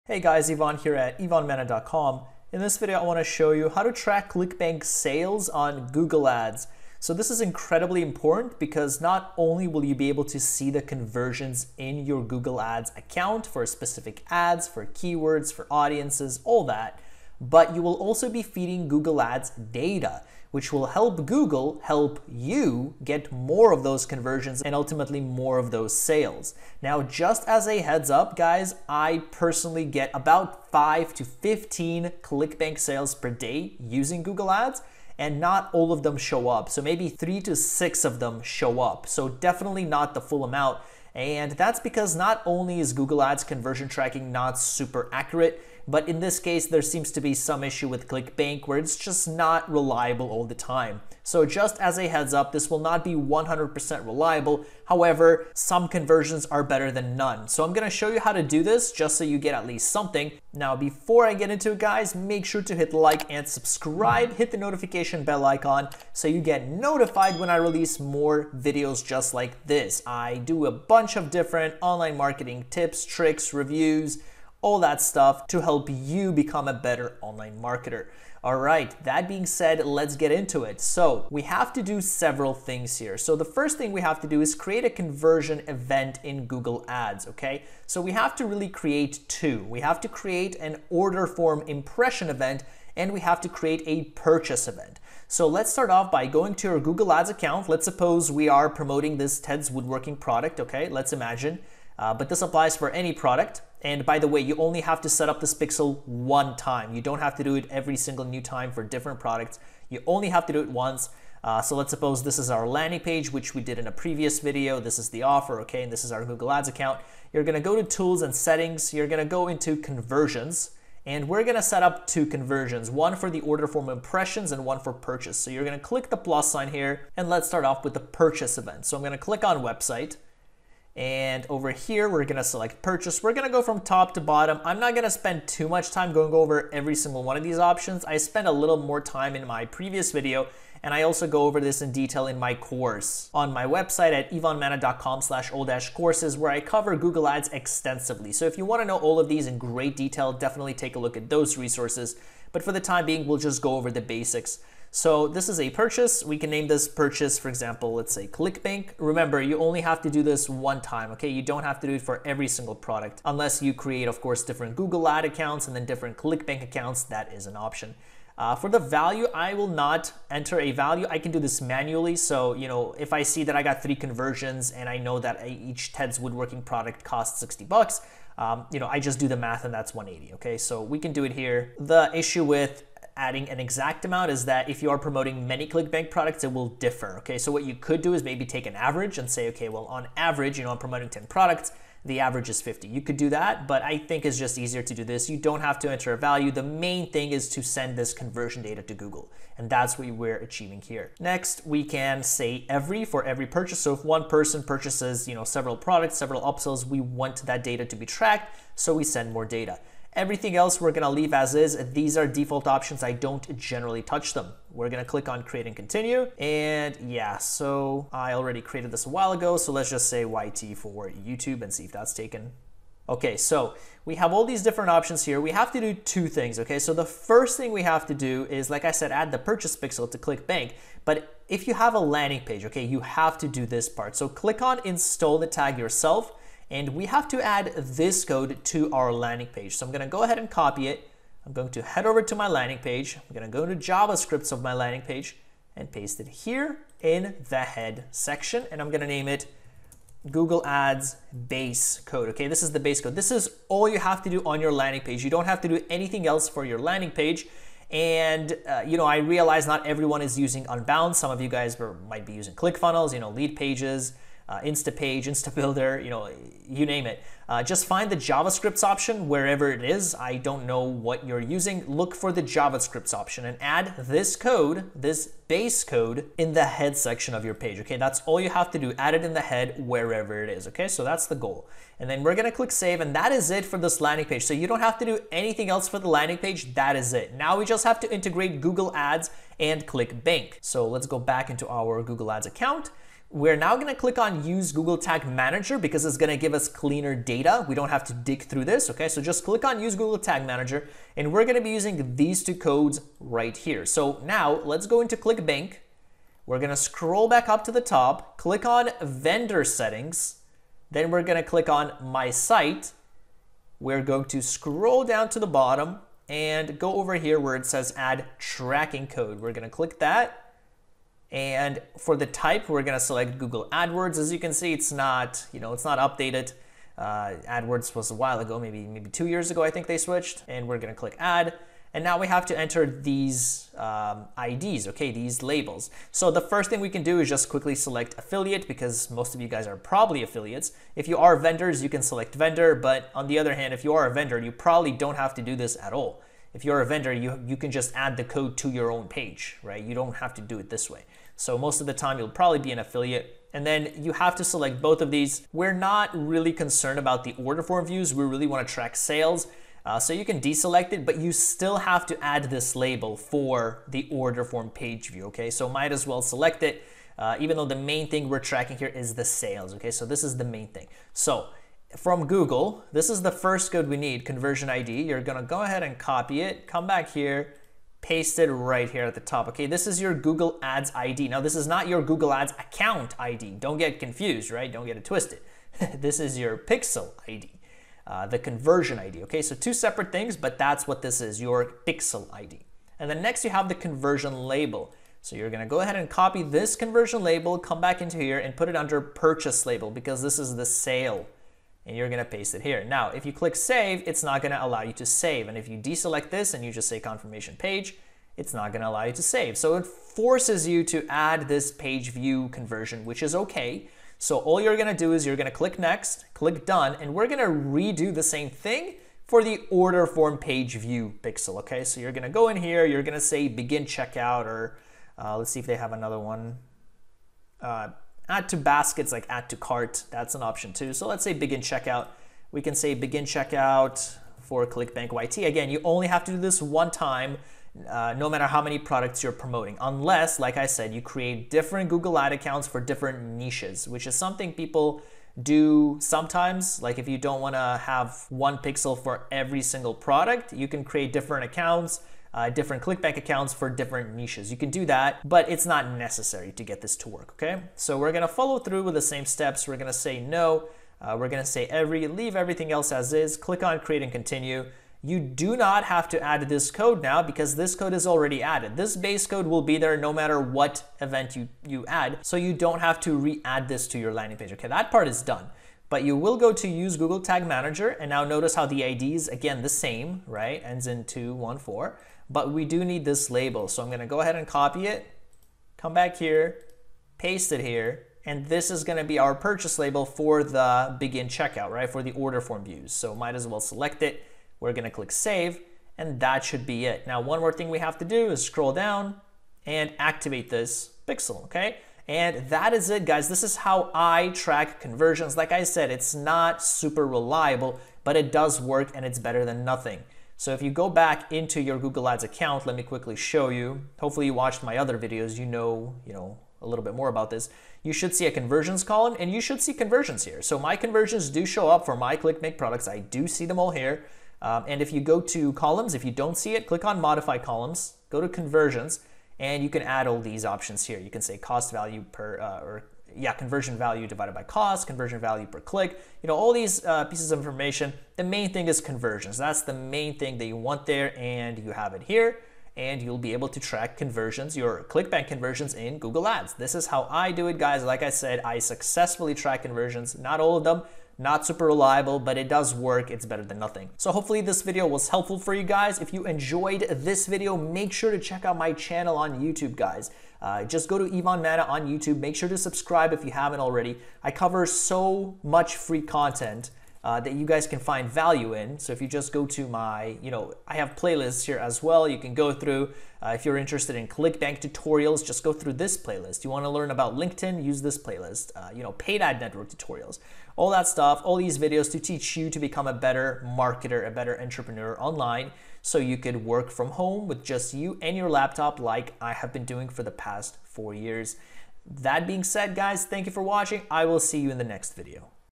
How to Track ClickBank Sales On Google Ads (Step-By-Step Tutorial)
Looking to track ClickBank sales on Google Ads in 2021?

I show you exactly how to do that in this step-by-step video so that you can see conversion data in your Google Ads account and feed this data to them so that they can get you even more sales.

Table of contents:

0:00 - Introduction
1:57 - Set up a Google Ads conversion event
3:59 - Add the code to your website
5:03 - Add the code to ClickBank

Step 1: Set up a ClickBank and Google Ads account

I assume you already have a ClickBank and Google Ads account set up, and if not, just go to clickbank.com and sign up as an affiliate - it is incredibly simple to do.

Then to sign up for a Google Ads account just head over to ads.google.com and create an account there.

Step 2: Set up a Google Ads conversion event

Click on 'tools' in the Google Ads interface and then click on "Conversions".

Click the little plus sign and select 'website', since we are trying to track clicks to our website.

Next, we should create a name for our conversion tracking to indicate what it is exactly that we want to track. In this case we're trying to set up sales tracking for ClickBank, so just select "Purchase" from the dropdown and give it a name for your reference.

You only have to do this ONE time for ALL your ClickBank products.

For the 'value' field, we need to insert a value for our conversion - how much is our conversion worth to us?

If it's for a sale that was actually completed, perhaps we can put in the full dollar value of that sale.

If your definition of conversion is something different, then you can create a value for that conversion to be whatever you see fit (for example everyone who signs up to my list is worth $xx to me).

Just keep in mind that this conversion value will apply to ALL your ClickBank products, so it's not as easy just placing any conversion number in there because it will change. So generally I leave it at "don't set a conversion value" because the main thing is to send conversion data to Google, not necessarily enter an exact sales amount.

For the 'count' value, we need to specify what we want to count as a conversion. If ONE person buys multiple products from us, is that one conversion, or multiple?

The conversion window just specifies what time frame you want your conversion to track.

Lastly, for 'include in conversions', we have to say yes. This is what determines whether Google will actively show the conversions in the columns on your ads bar to determine what ads/ad groups/campaigns work the best.

Once you're done filling in the blanks, click on "Save" and you select the "Install the Tag Yourself" option.

We're going to use this in this next step of tracking ClickBank sales on Google Ads!

Step 3: Add the code to your website

If you have a website (and you should if you're promoting on Google), you have to add this BASE code to the head section of your landing page.

Whether you're using ClickFunnels, Unbounce, Leadpages, or any other platform, just take this code and add it to the head section of the page.

This is important if you want to track ClickBank sales on Google Ads. Let's move on to the next step.

DO NOT click on "Save" yet - we still need this last conversion event page in Google for the next step.

Step 4: Add the code to ClickBank

You will then head over to ClickBank, go to 'vendor settings', then 'site', and scroll all the way to the bottom where you see the integrated sales reporting tab.

Select "Google AdWords" from the dropdown, then click on 'add new', or 'edit', and copy your Google ID from the 'google id' description you just pasted into your notepad. For the order form, input the 'google conversion label' series of numbers inside here. Do not copy the quotation marks though.

In your Google Ads account, select the "Google Tag Manager" tab and copy the Google pixel ID and the conversion label and paste it in the corresponding fields in ClickBank.

If done correctly, you will see a blank "Page View Label" field.

To fill this in, you have to click "Save", then go back and redo steps 2 and 4 but this time for the order form views conversion event, NOT the purchase event.

Once you do that, you should start seeing sales in your Google Ads account under the "Conversions" column. It's not very reliable, but it's better than nothing.

And this is how you set up ClickBank sales tracking on Google Ads in 2021!